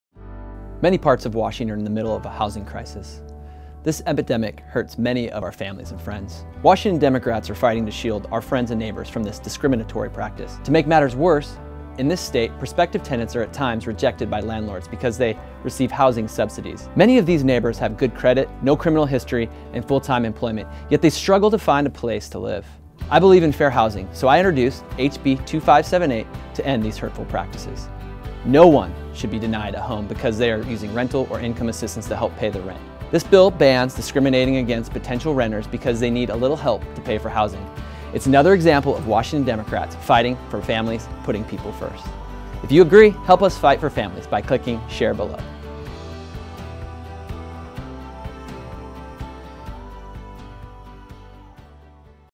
Ending discriminatory housing practices
Washington is experiencing a housing crisis. Families are struggling to find a place to live and income discrimination has made matters worse for those who receive housing subsidies.

Rep. Marcus Riccelli is standing up for these families by fighting to end housing discrimination against those who need a little help to pay the rent. Everyone deserves a place to call home, and Democrats are putting people first in the fight for fair housing.